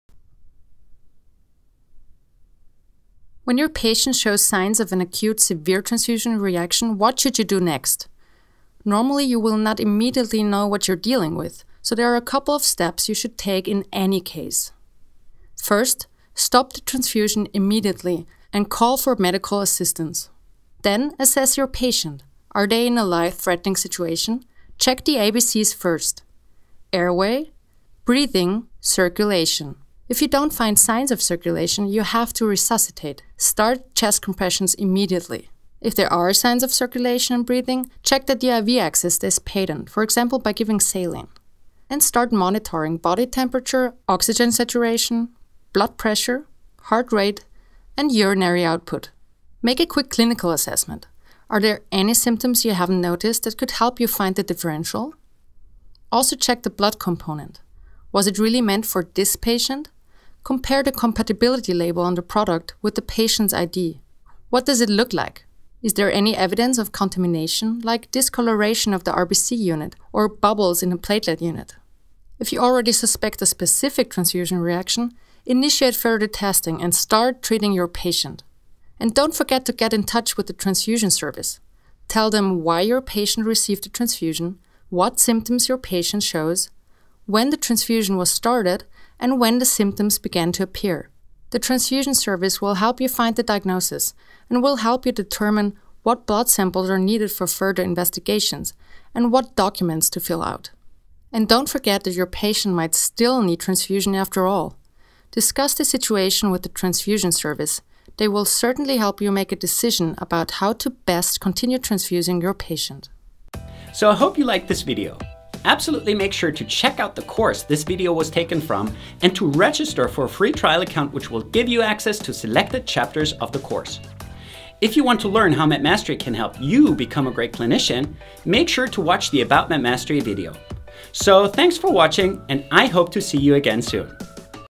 Acute Transfusion Reactions: Management Techniques
Prepare for the unexpected with strategies to handle acute severe transfusion reactions. This video covers essential management steps for critical situations during or after transfusions.